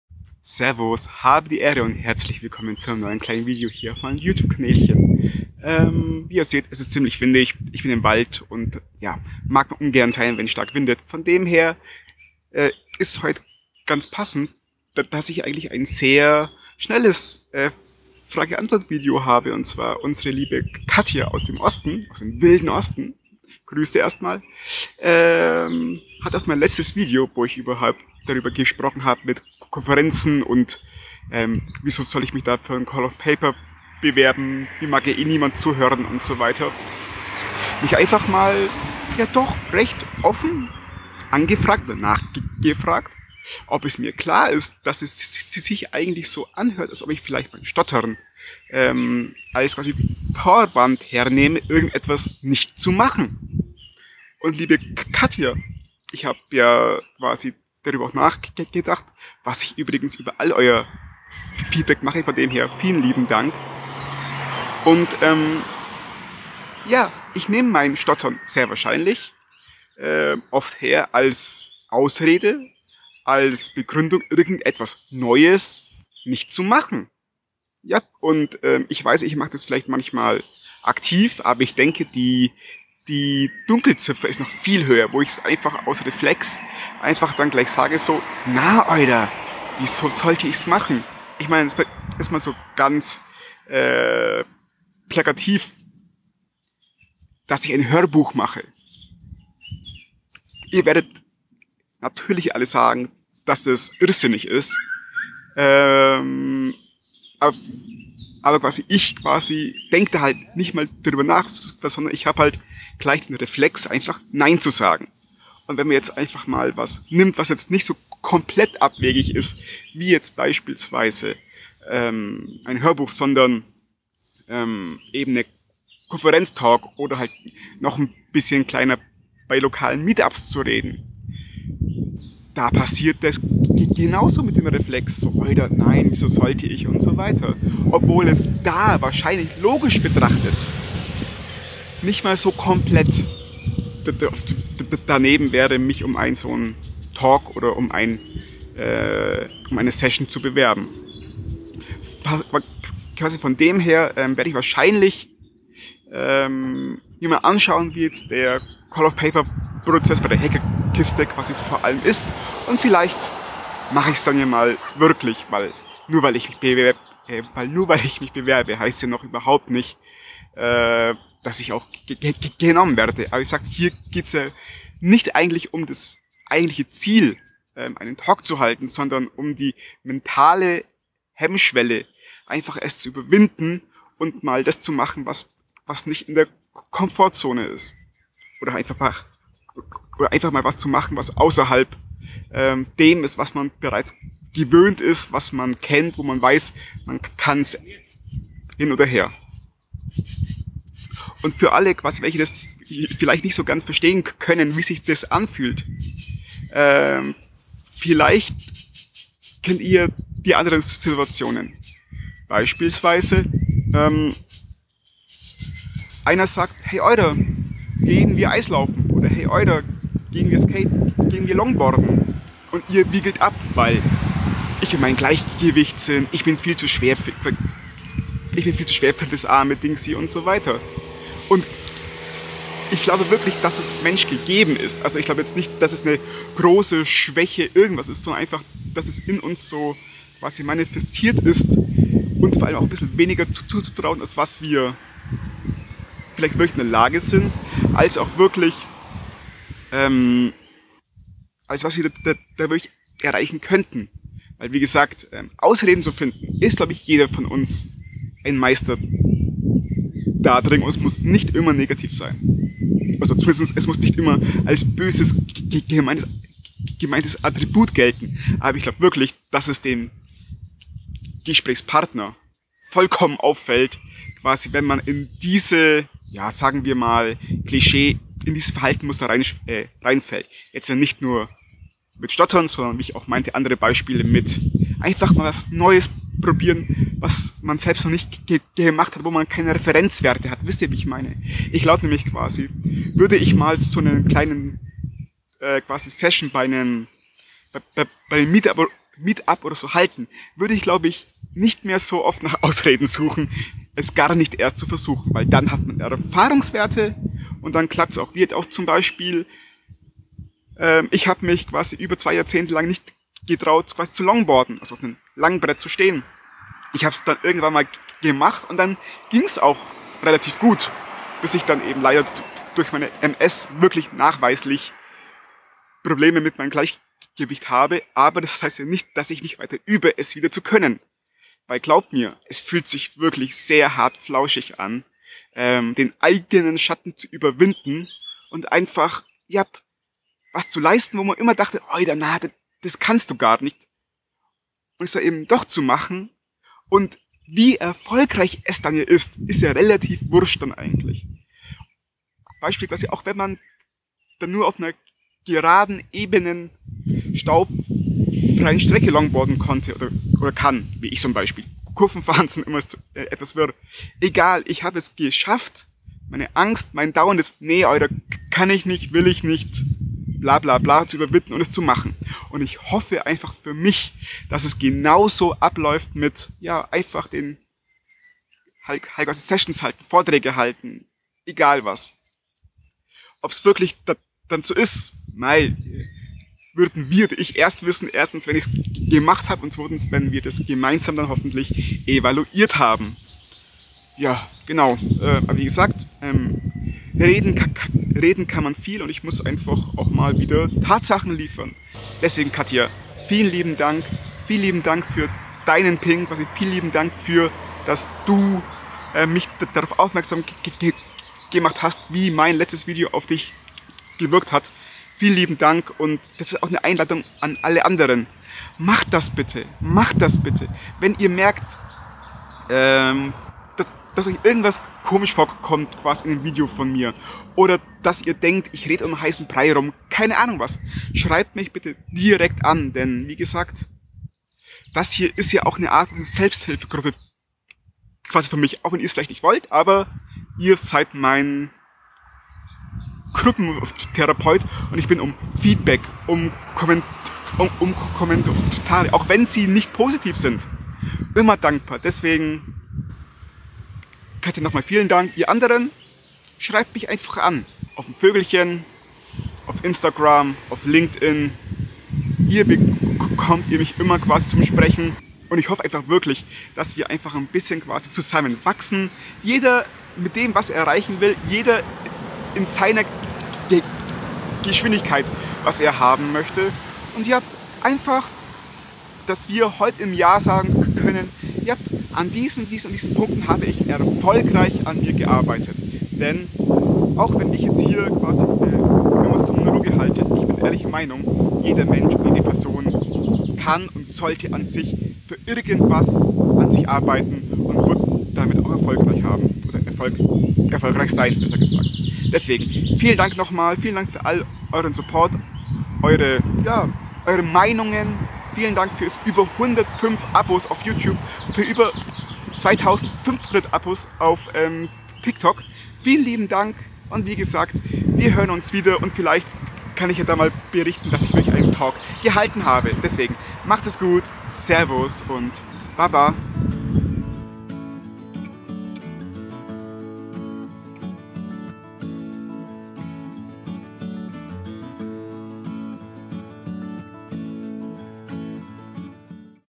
Na, Oida! Nutze ich mein Stottern als Ausrede? Achtung: Windgeräusche
Servus und Habedieehre und schön, dass Du da bist!

Vielen Dank für euer Feedback! Heute gehe ich auf die Nachricht von Katja ein. Diese handelte von der Frage ob ich mein Stottern auch als Ausrede nutze - wenn ich nicht absichtlich.

Wie ich zu dieser Aussage stehe, erfahrt ihr in diesem Video.

So oder so, vielen Dank für euren großartigen Support! Es ist so awesome wie ihr mich motiviert.
Merci!

Ihr seid großartig!